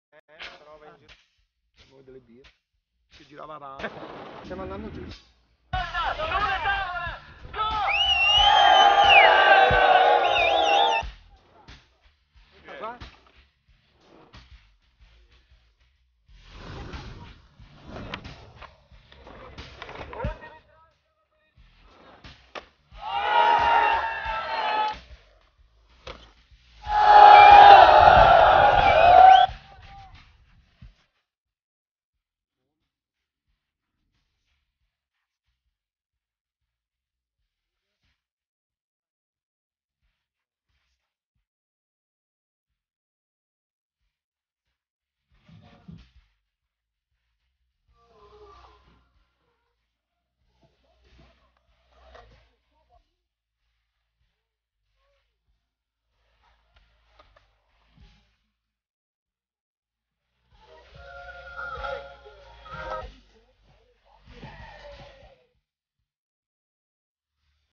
Redbull Skate Week Milano - Go Skate Day
*THE ORIGINAL MUSIC HAS BEEN REMOVED BECAUSE OF COPYRIGHT REASONS*

After a month here's the video I shot with VFL, Jurij Girotto & Marco Mestre at the Redbull Skate Week on the 21st of June 2018. I hope you like it!